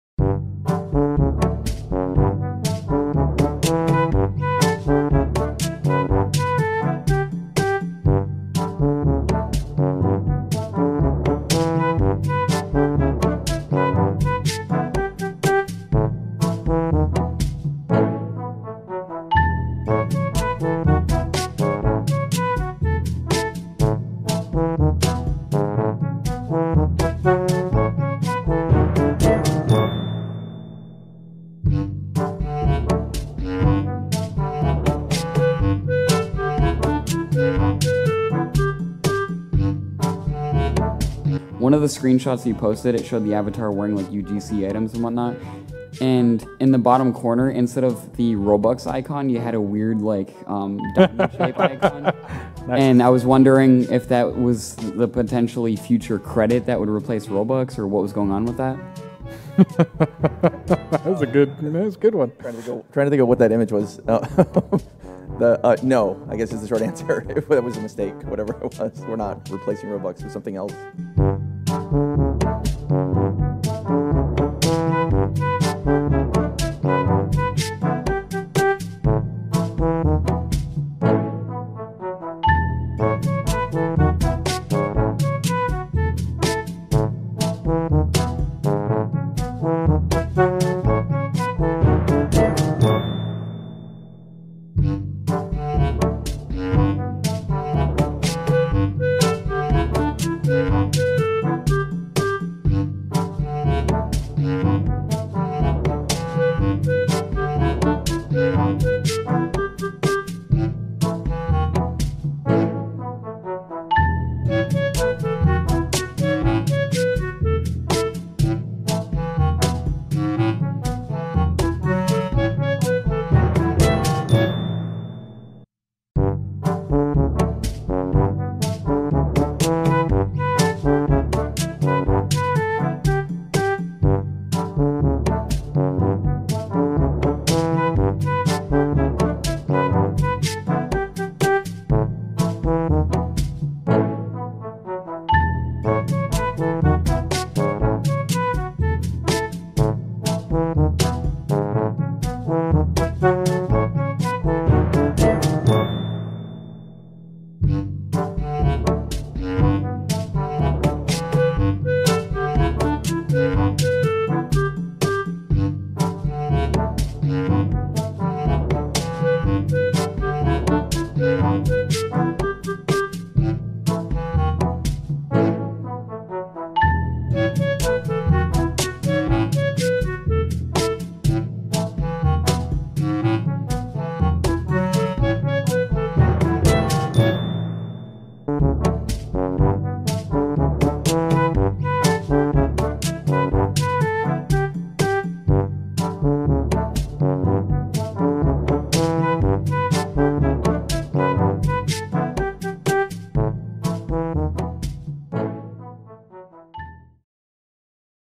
Roblox is REPLACING Robux?
A new Robux logo popped up which as caused people to think Roblox is replacing Robux... could it be true?